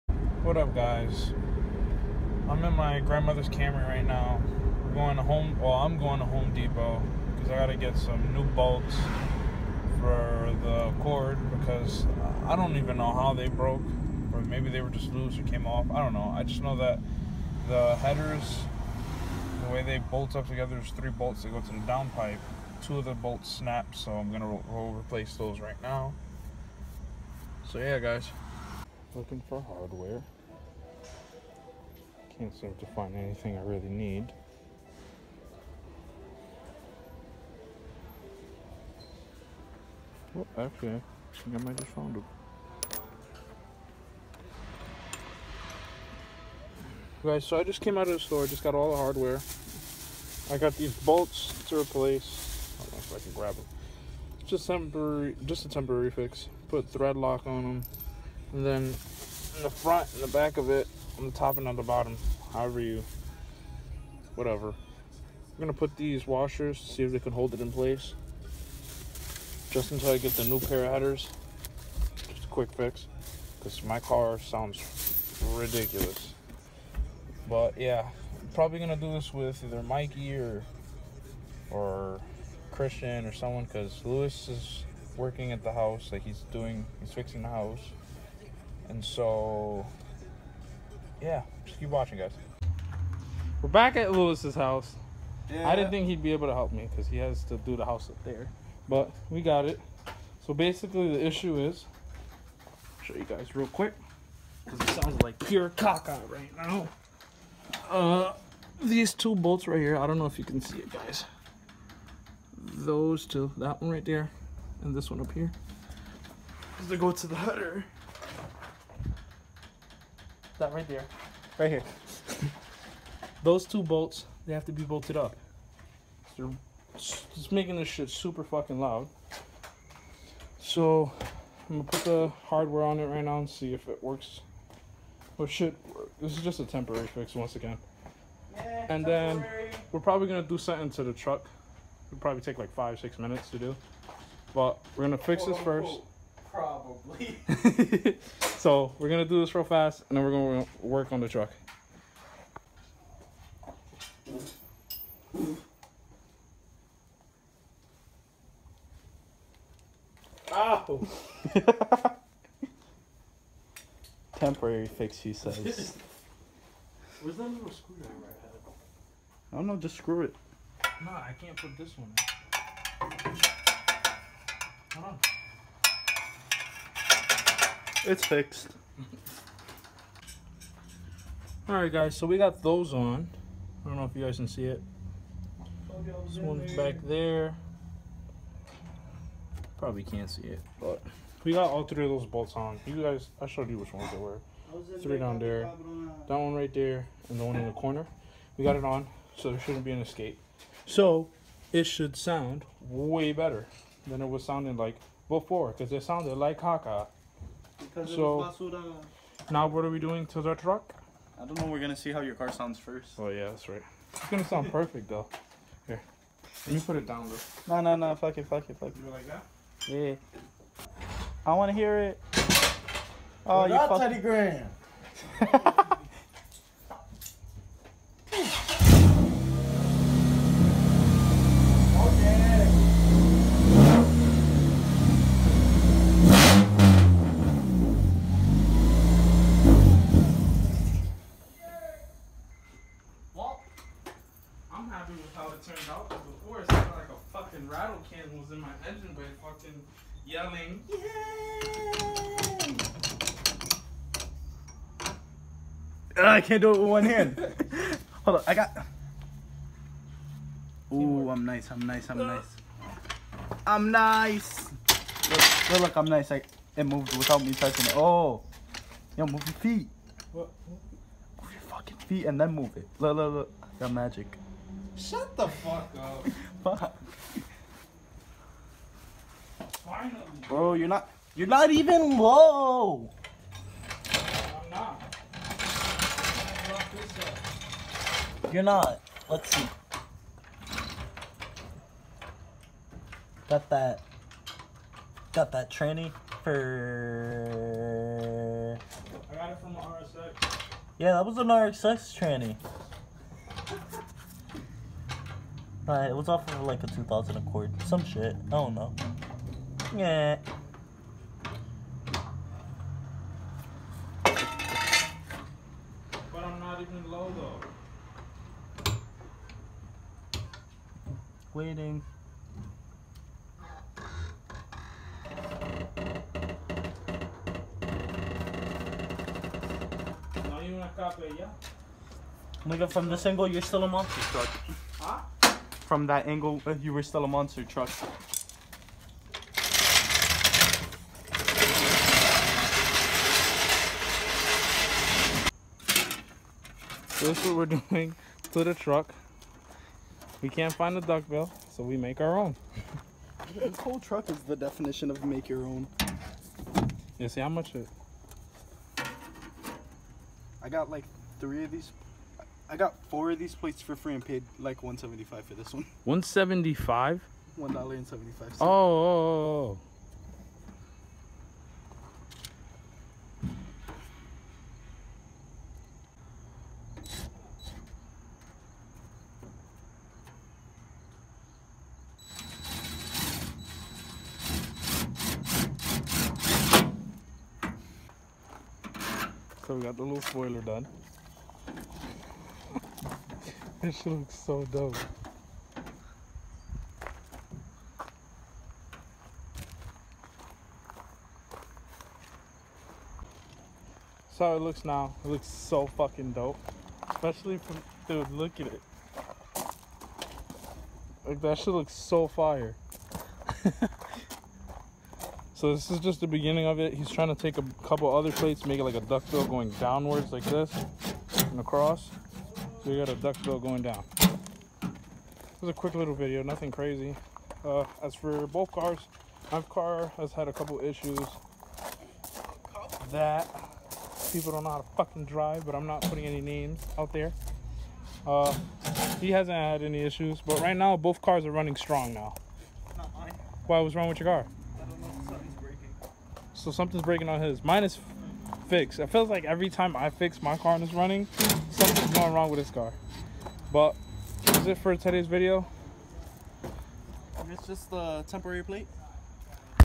Scrape Gang | accord breaks again and the mini truck gets a dope duck bill
in todays video The bolts that hold the headers together broke off and were making the Accord sound like crap. luis got creative and made his own duck bill out of license plates.